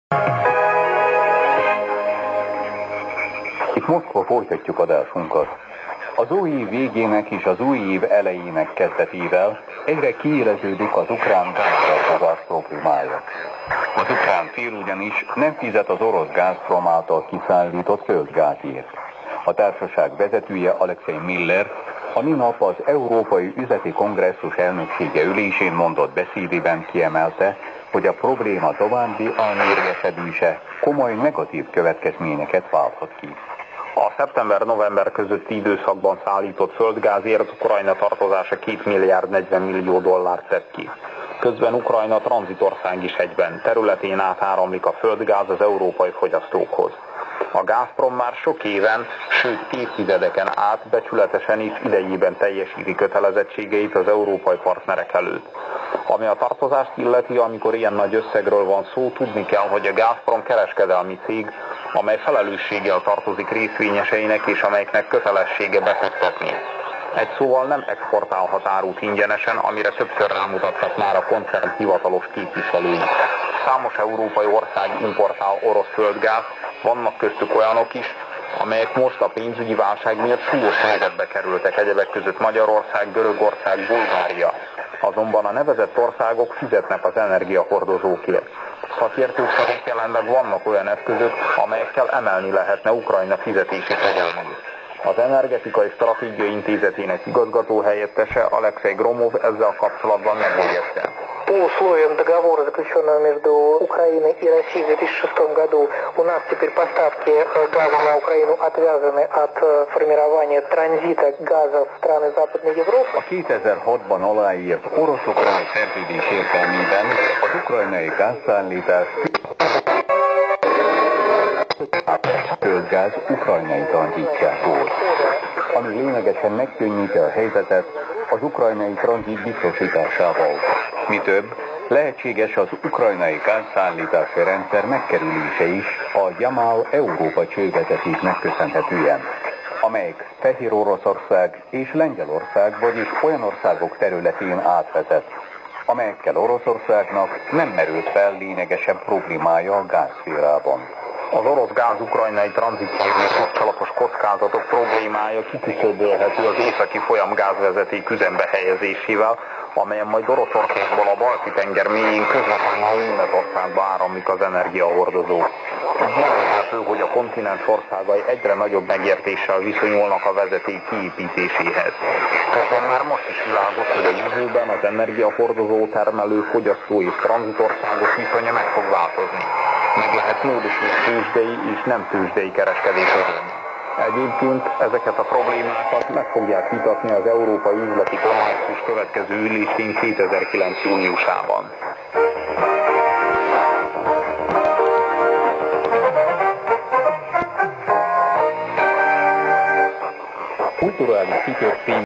Oroszország Hangja Rádió - magyar nyelvű adás a 6025 KHz-en 3. rész
orosz propagandaadás magyarul
Voice Of Russia in Hungarian